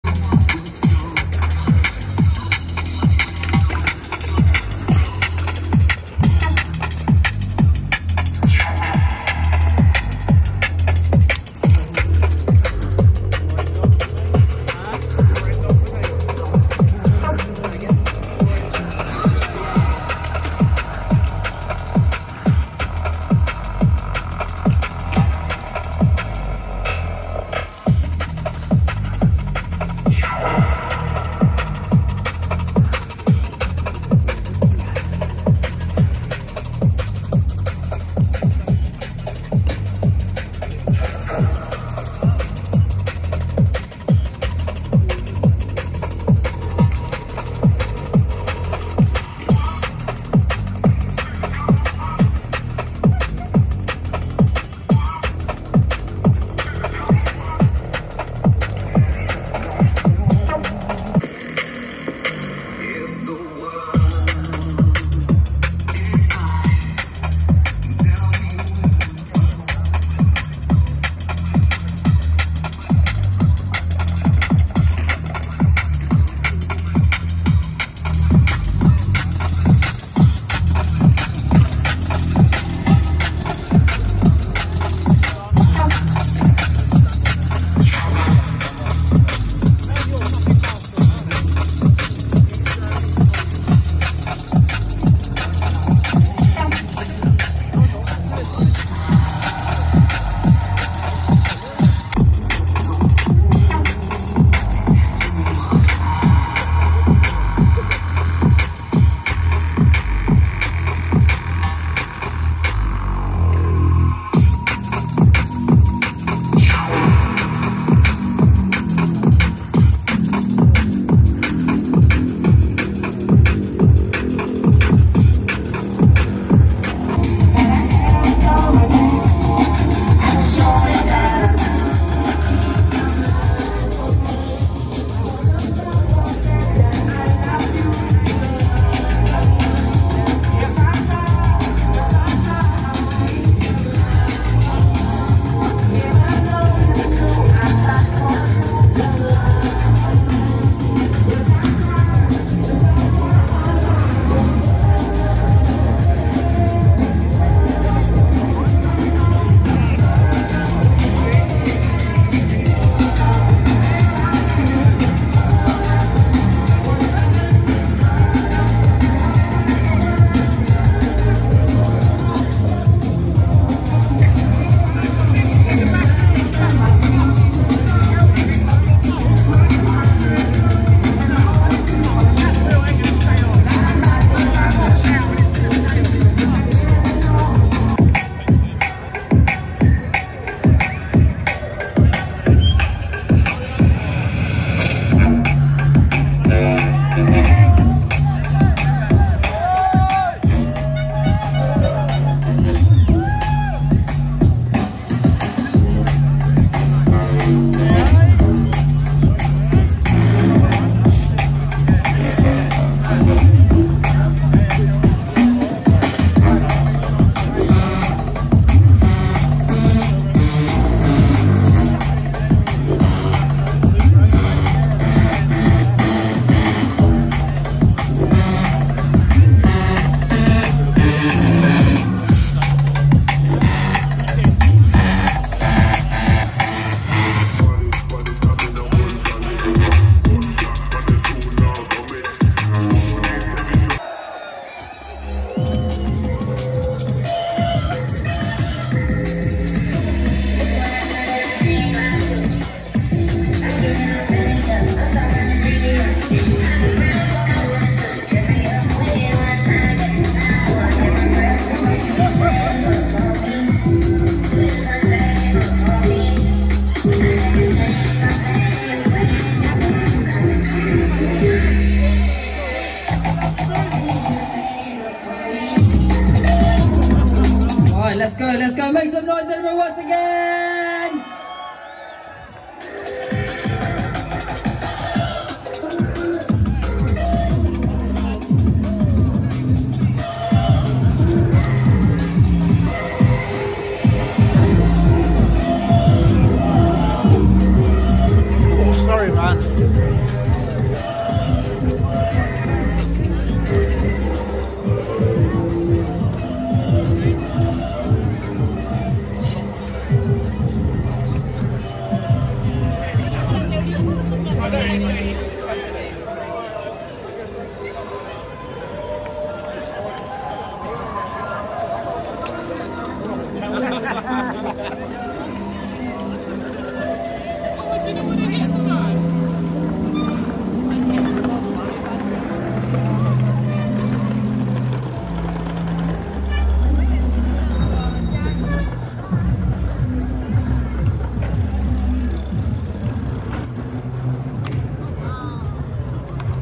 DRUM & BASS ON A BIKE. EDINBURGH. DOM WHITING. WHAT'S NOT TO LIKE. YES YES YES.
Dom Whiting pedalled into Edinburgh on Sunday with his all conquering D&B on a BIKE.  The sun was out and so were the bikes.
This is one of those.  It really restores one's faith in humanity.